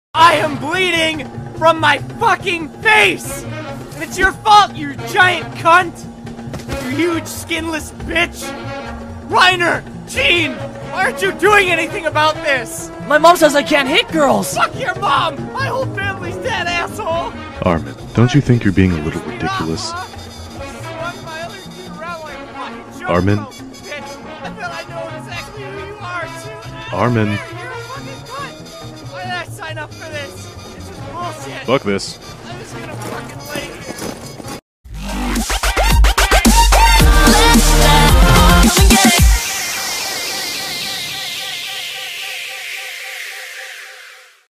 [EShorts #4] Attack on Emberstar
:EDIT: I know I zoomed to Knockpelt when it said Reiner ;w; I'm sorry too late to fix that

Breakclaw unleashes his inner titan-killer on Emberstar... with the expected results...

Characters:
Breakclaw
Emberstar
Knockpelt
Stonestar

Original idea by Tei :3